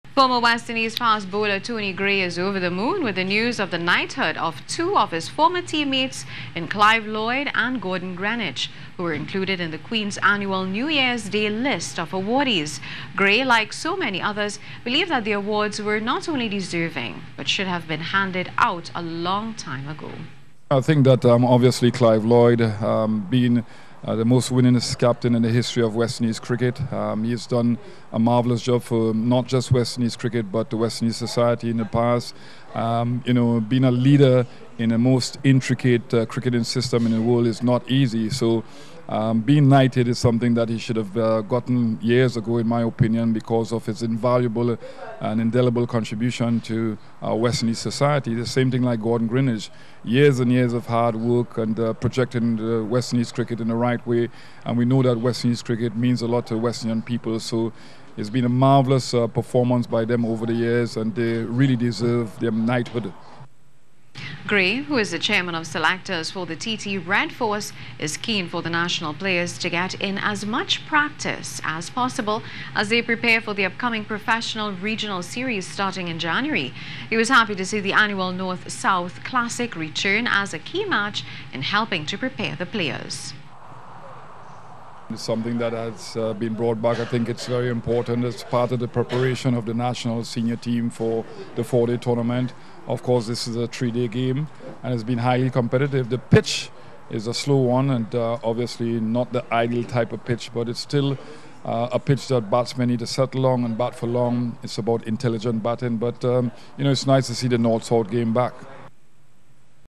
SPORT: Gray Happy For Lloyd & Greenidge
Former West Indies fast bowler Tony Gray is over the moon with the news of the knighthood of two of his former teammates in Clive Lloyd and Gordon Greenidge, who were included in the Queen's annual New Years's Day list of awardees.

Gray, like so many others, believe that the awards were not only deserving but should have been handed out a long time ago.
Gray, who is the chairman of selectors for the TT Red Force, is keen for the national players to get in as much match practice as possible, as they prepare for the upcoming professional regional series starting in January.

He was happy to see the annual North/South classic, return as a key match in helping to prepare the players.